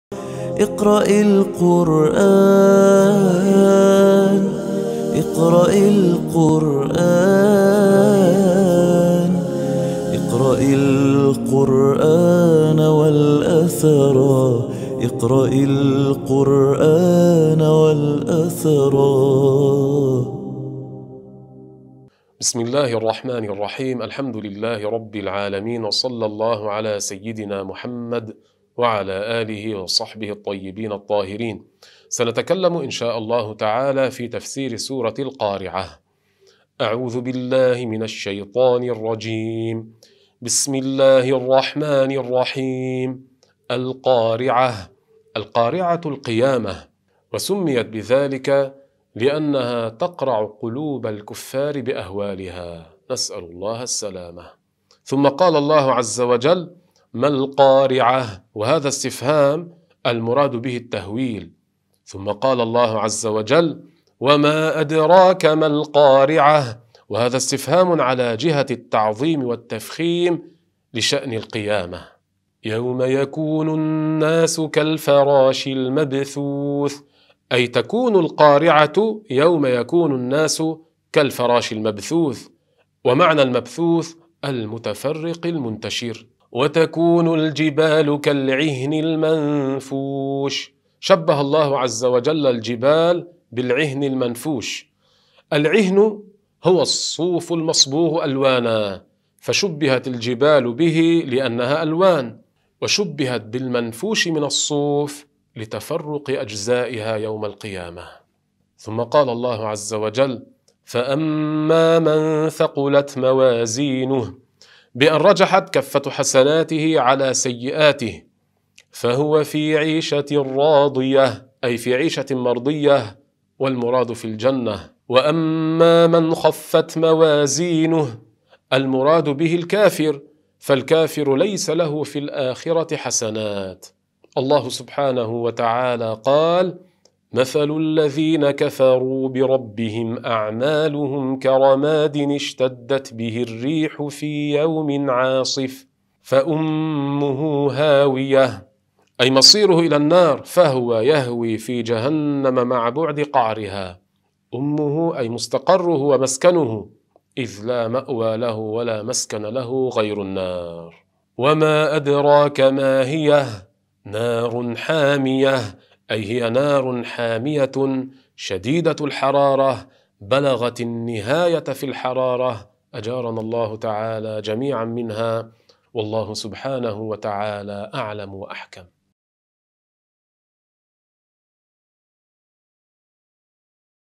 تفسير سورة القارعة
تفسير سورة القارعة مع فضيلة الشيخ رمزي شاتيلا